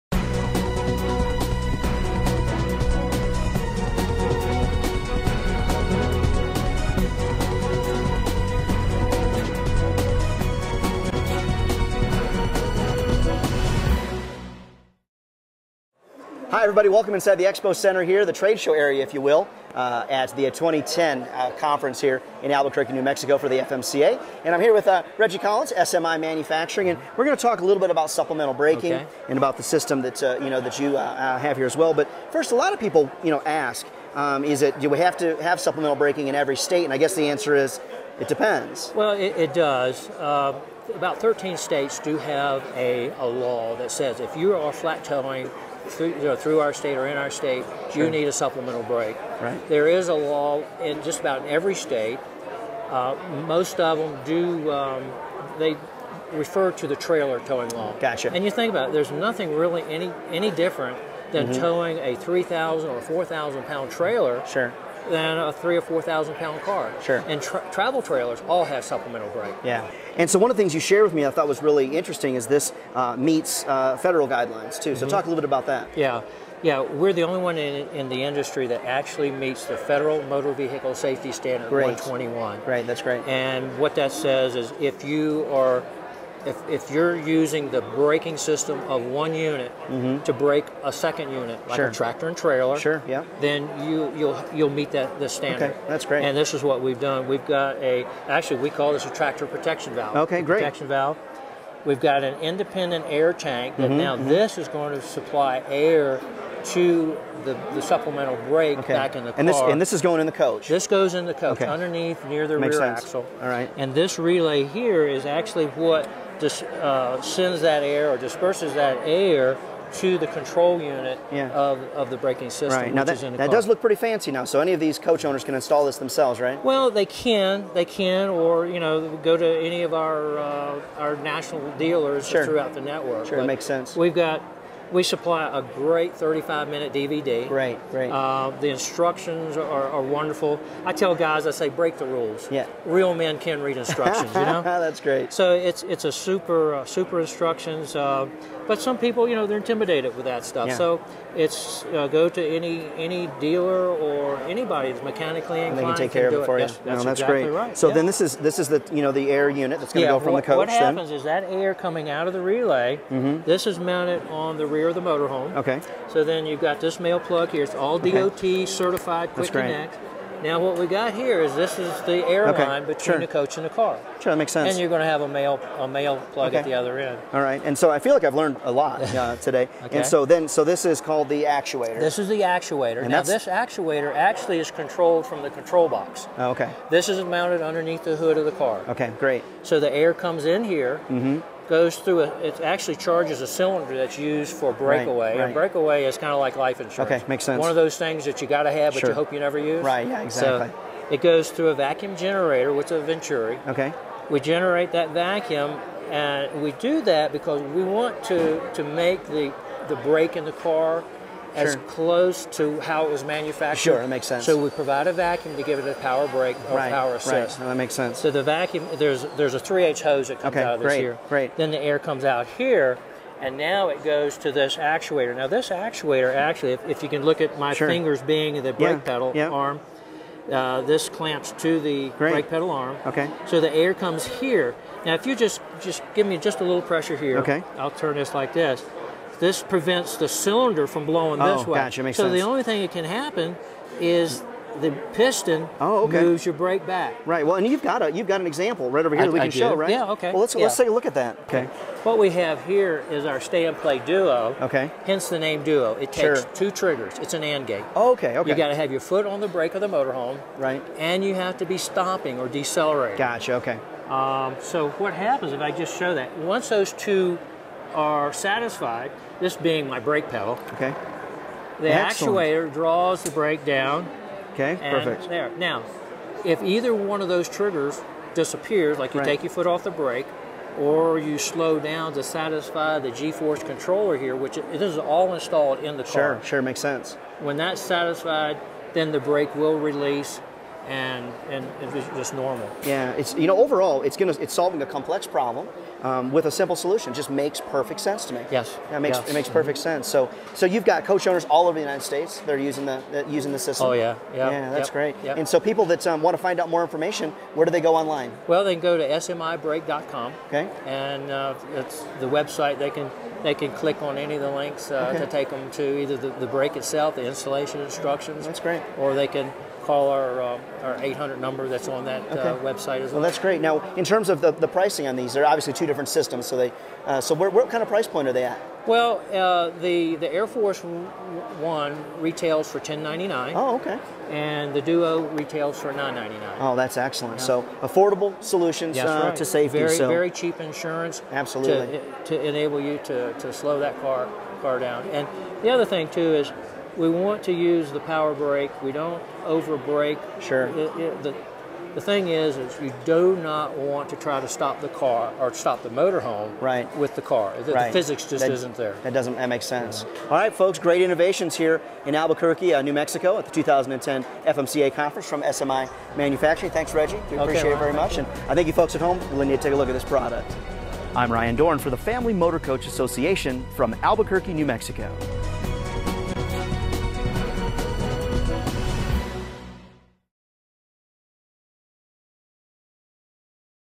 SMI's Air Force One Towed-Vehicle Braking System
A close-up look at SMI Manufacturing's Air Force One supplemental braking system -- how it works, installs and the technology behind it.